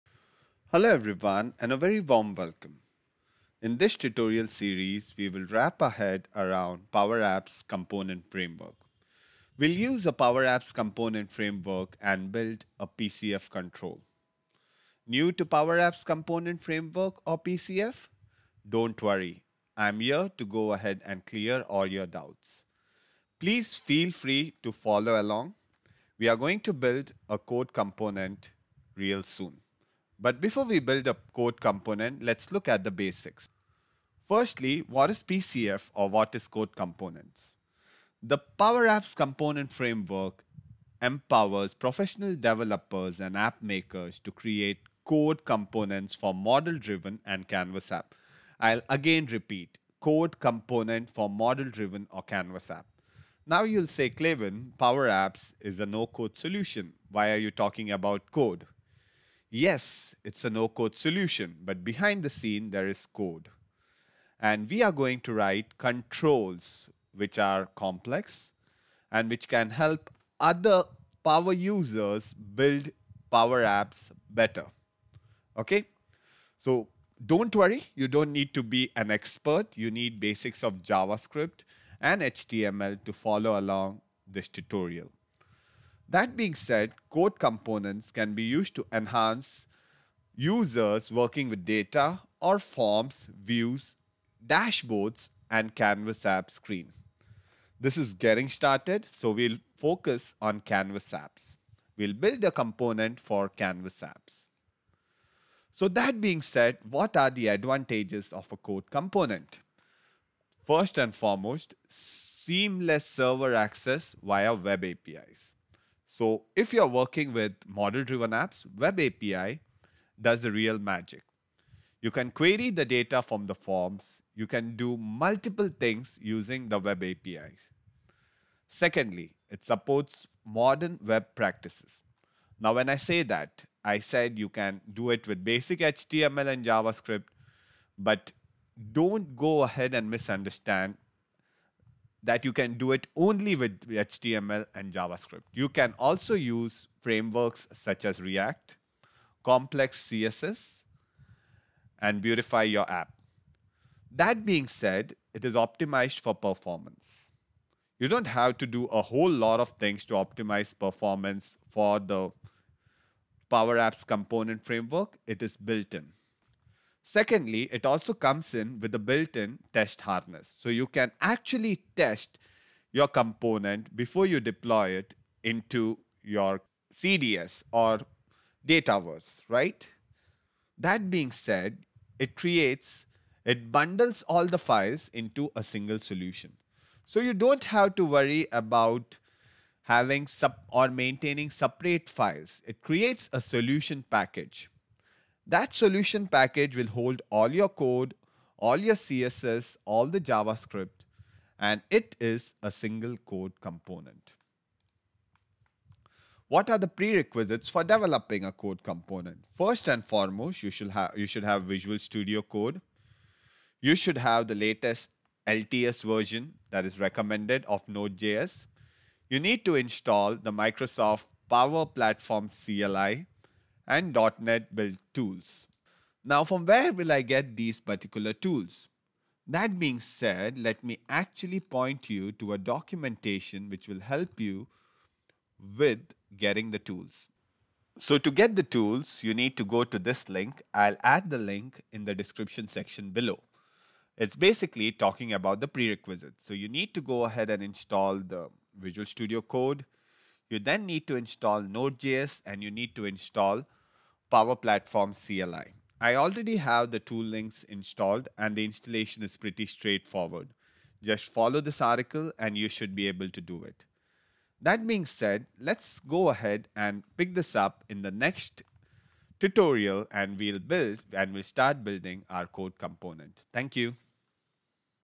Getting Started with PowerApps Component Framework Tutorial 1
Power Apps component framework empowers professional developers and app makers to create code components for model-driven and canvas apps. These code components can be used to enhance the user experience for users working with data on forms, views, dashboards, and canvas app screens.